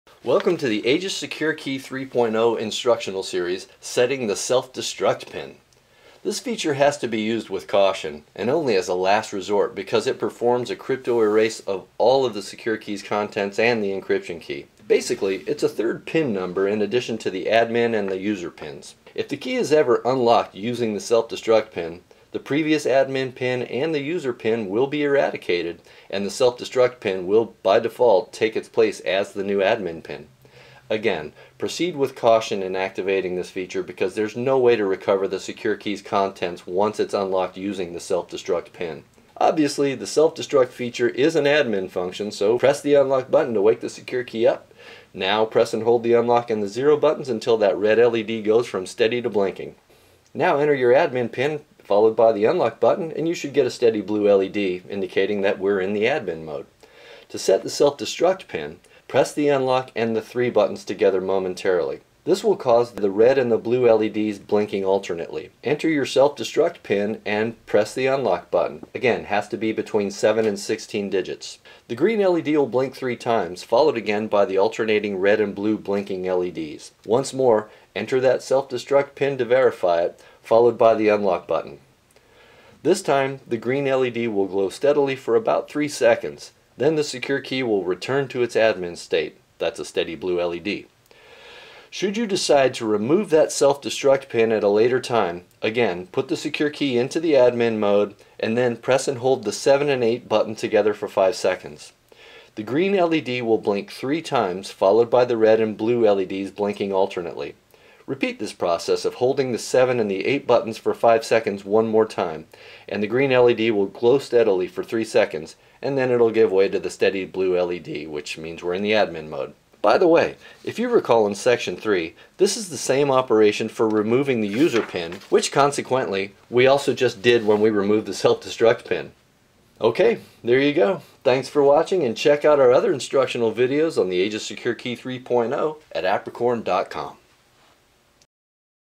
Aegis Secure Key 3.0 Tutorial 7: SETTING THE SELF DESTRUCT PIN
Aegis Secure Key 3.0 instructional video series covers how to establish the self-destruct PIN, as well as how to return it to its "off" default position.